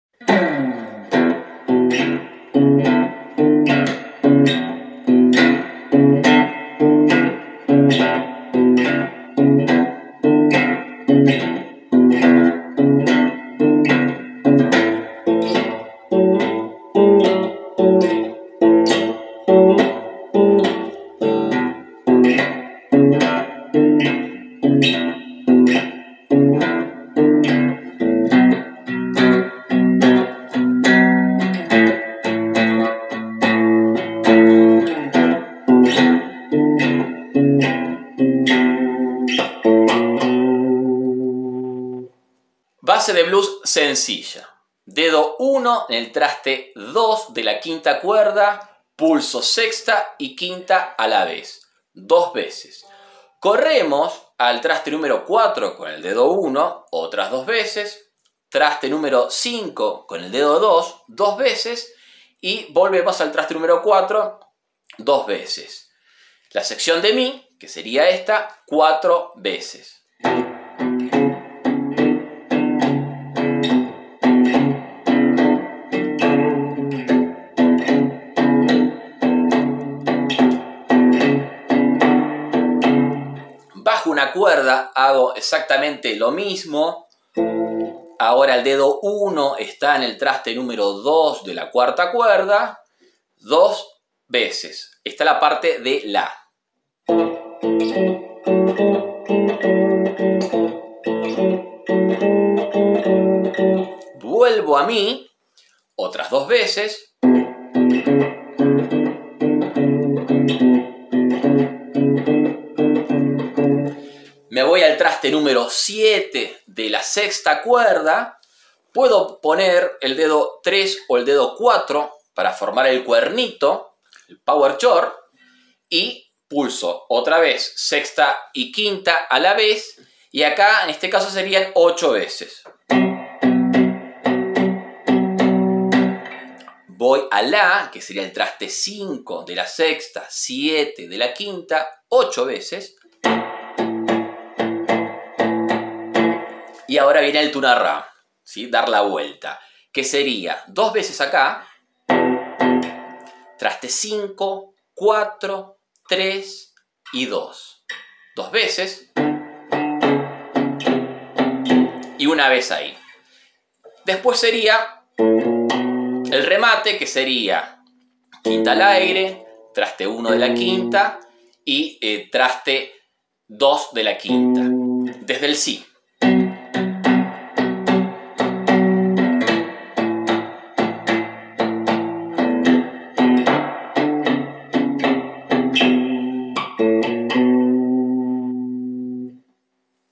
Base de Blues sencilla. Tutorial fácil para Guitarra.
Base sencilla de Blues en E para guitarra. Tutorial paso a paso.